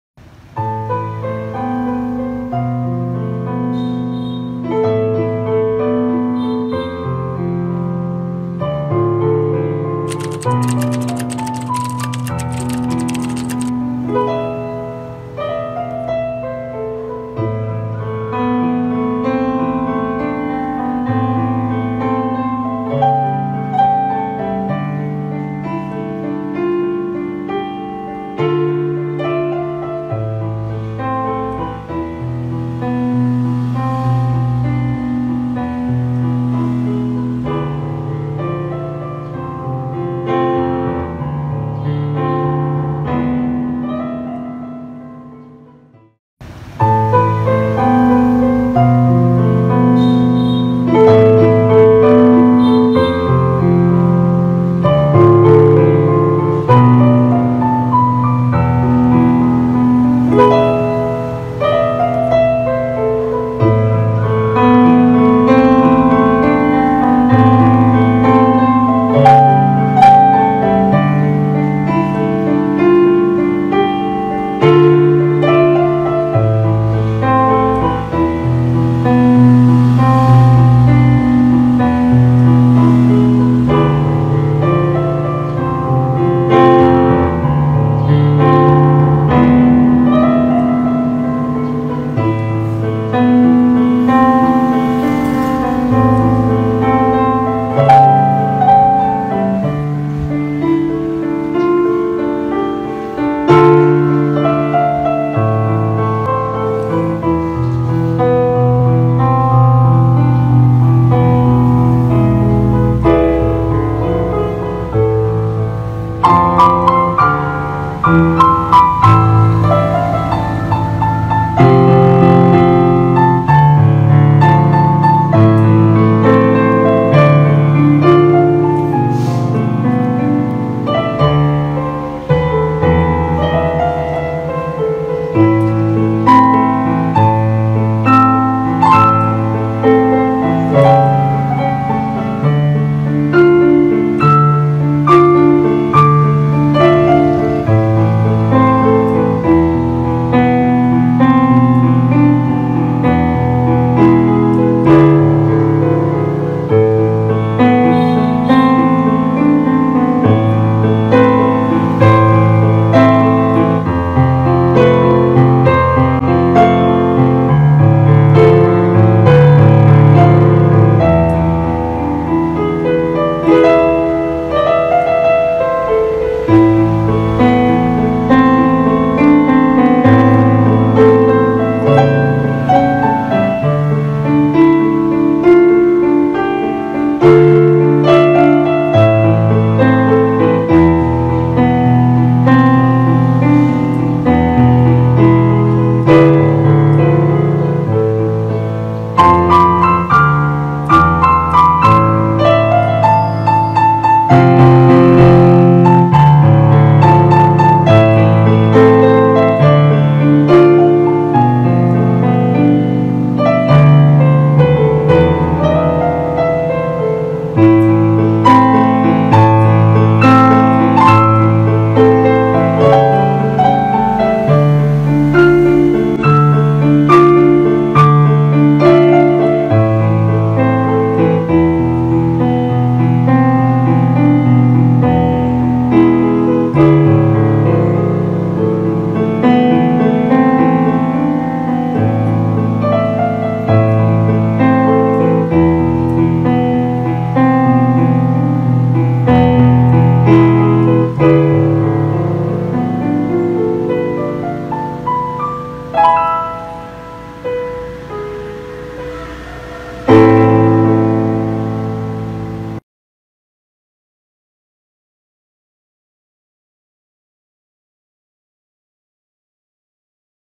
[MỚI NHẤT]NHIẾP ẢNH NHÍ NGÔ NGUYÊN KHANG VIẾNG MỘ CỐ NS TRỊNH CÔNG SƠN VÀ ĐÀN PIANO CHO ÔNG NGHE
NHIẾP ẢNH NHÍ
NGÔ NGUYÊN KHANG
VIẾNG MỘ CỐ NS TRỊNH CÔNG SƠN
VÀ ĐÀN PIANO CHO ÔNG NGHE
PROMOVIES VIDEOKAR NS QUANG DAU